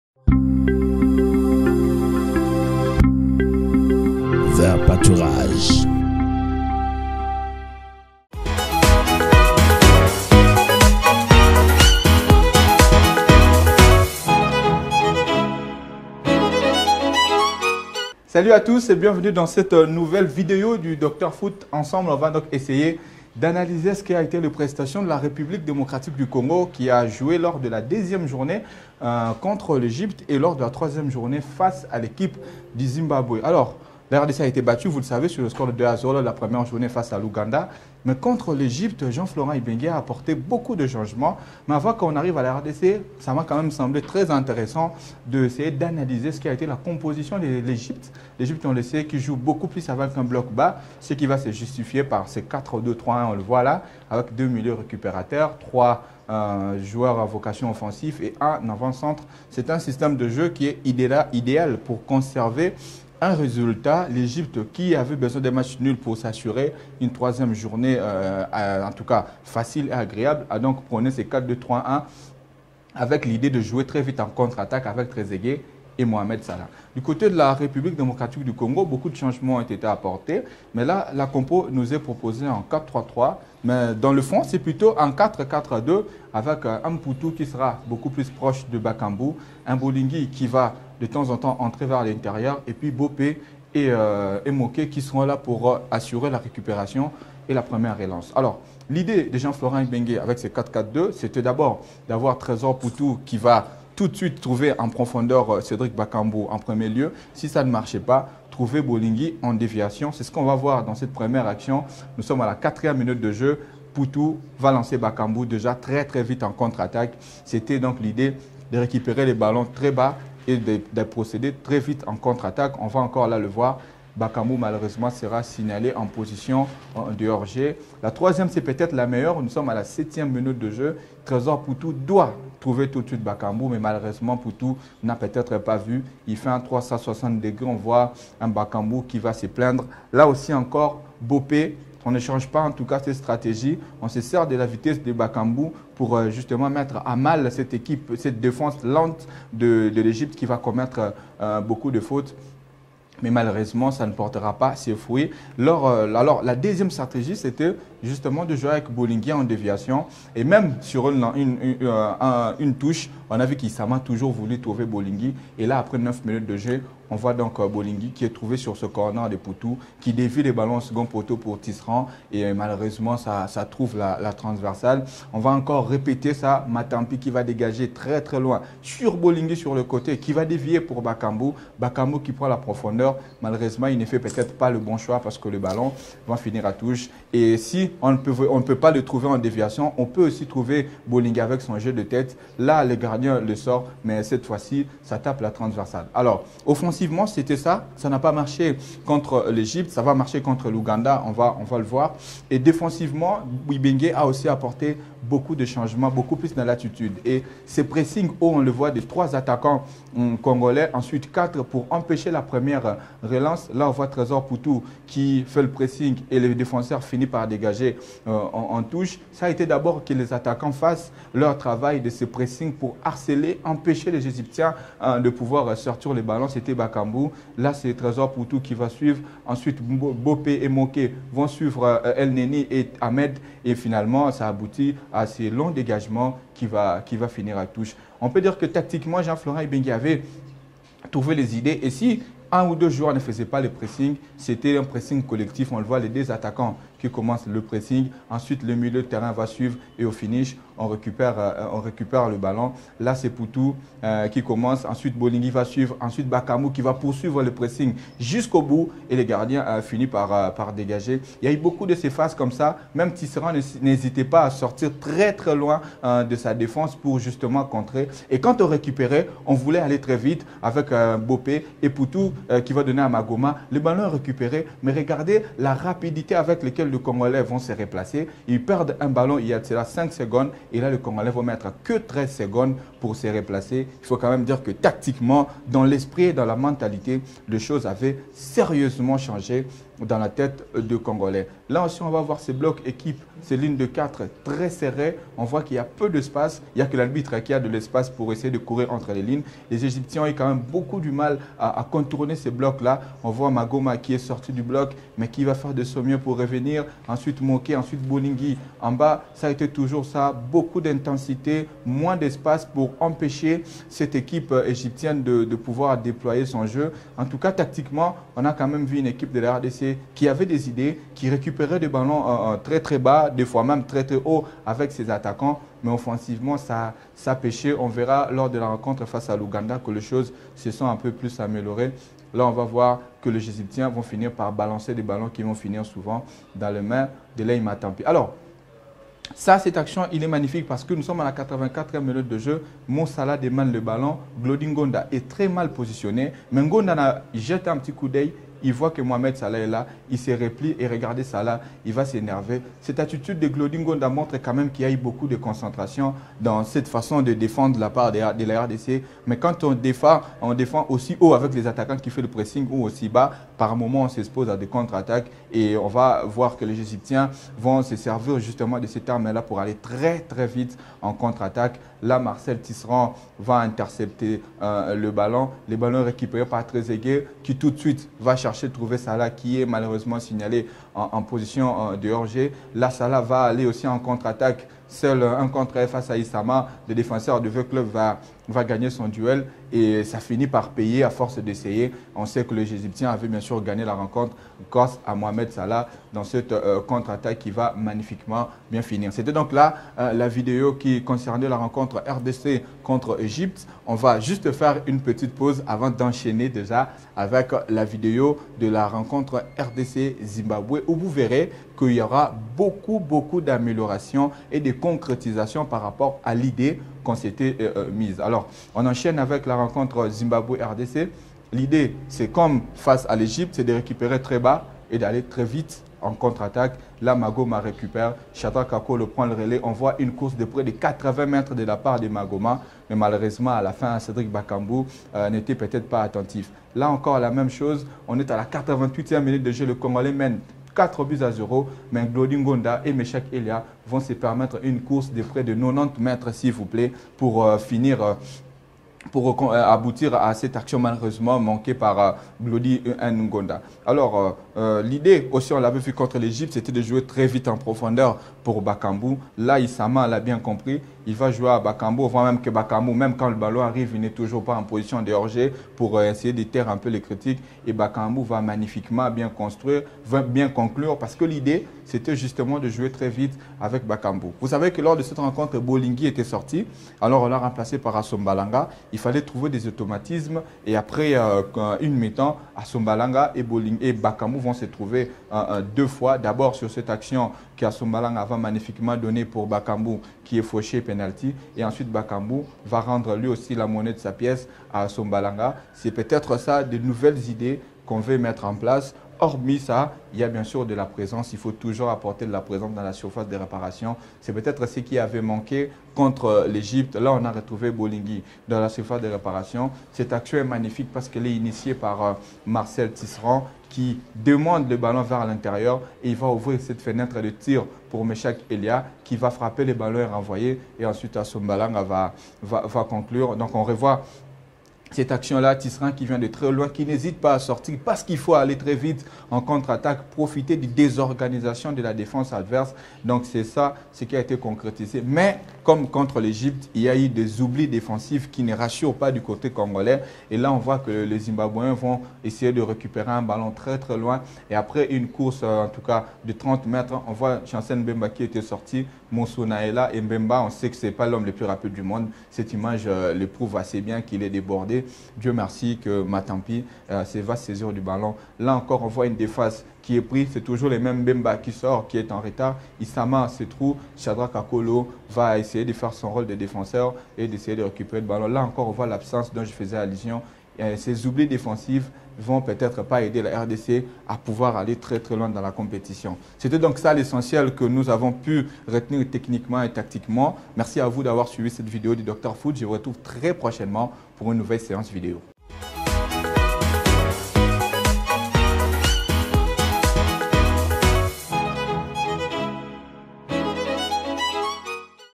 Analyse des matches RDC vs EGYPTE & RDC vs ZIMBABWE
Docteur foot est une émission footballistique qui  a pour objectif l'analyse tactique des rencontres de foot en RDC avec palette a l'appui.
présenté par Chadrac NTENTA.